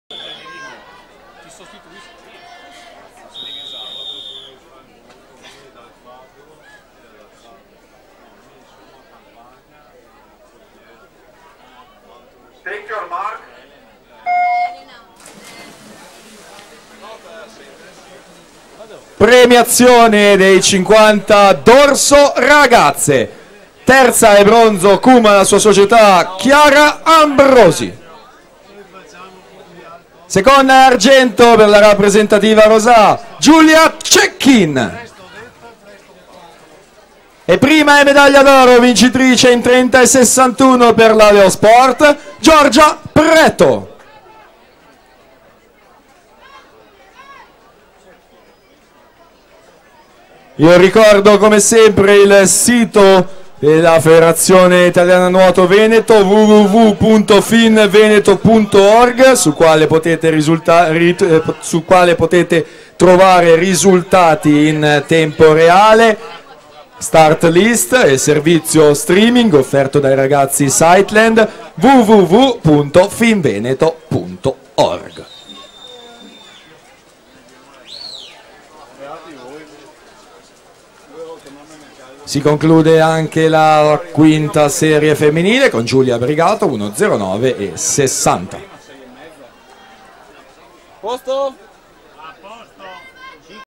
100 Stile Libero Esordienti A Femminile (Serie 5) - 2 Treviso Swim Cup
Atleti partecipanti:
in corsia 1 TROTTA CLAUDIA (2005) della soc. STILELIBERO SSD iscritta con 01:12.90
in corsia 2 MESTRONI FRANCESCA (2004) della soc. ARCA TEAM iscritta con 01:12.00
in corsia 3 ALBERTI CHIARA (2004) della soc. AQUAPOLIS iscritta con 01:11.71
in corsia 4 MENEGALDO EMMA (2004) della soc. ASD ARCA NUOTO iscritta con 01:11.00
in corsia 5 BRIGATO GIULIA (2005) della soc. ANTARES NUOTO iscritta con 01:11.02
in corsia 6 CAUSIN VALENTINA (2004) della soc. STILELIBERO SSD iscritta con 01:12.00
in corsia 7 TIRALONGO FELICIA LIUDMILA (2004) della soc. AQUATIC CENTER iscritta con 01:12.00
in corsia 8 BETTARELLO GIULIA (2004) della soc. PIAVE NUOTO iscritta con 01:13.00